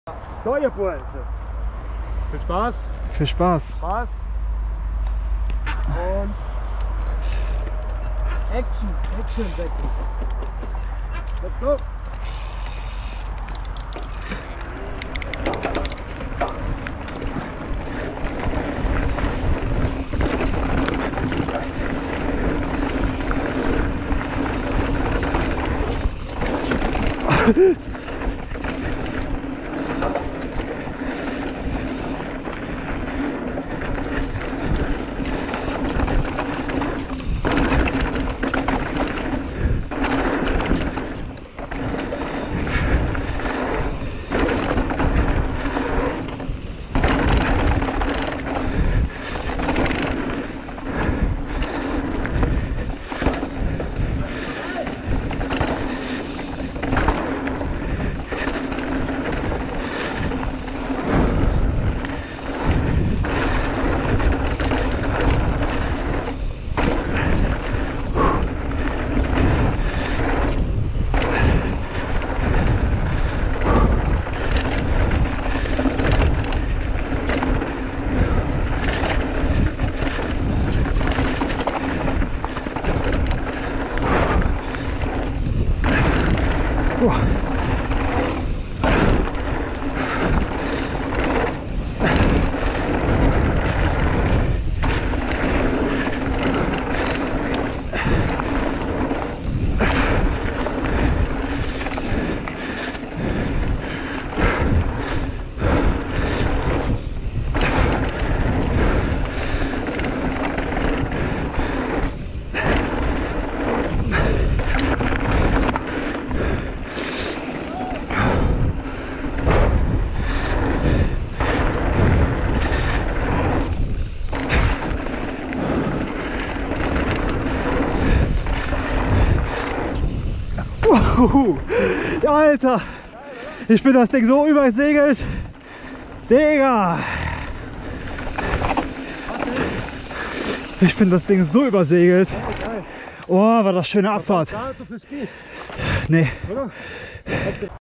TRAILPARK Mehring 🌞 E-MTB Run 💥Kenevo💥 Canyon Sender💥Trek Slash 💯Melo u. Max RAW🌞4k RAW
Dieses Video ist ein kurzer Clip über Die rote Line Jumpline im Trailpark Mehring zusammen mit meinen BIKEBUDDYS Melo und Max.
Ich liebe es im Winter Video zu schauen die Sommerliche Vibes versprühen, daher ist es eine willkommende Abwechslung zum momentanen Shitwetter.
Mein Specialized Enduro ist schon mega heiss auf 2022.
Demnächst werden frische Videos produziert, da diese mit etwas mehr Aufwand verbunden sind, kann ich den Wöchentlichen Rhythmus nicht immer einhalten.
Ich wünsche euch viel Spass, und ich hoffe der Frühling lässt sich nicht allzu viel Zeit dieses Jahr.

Euer Lars

Mein Equipment

Go Pro Hero 8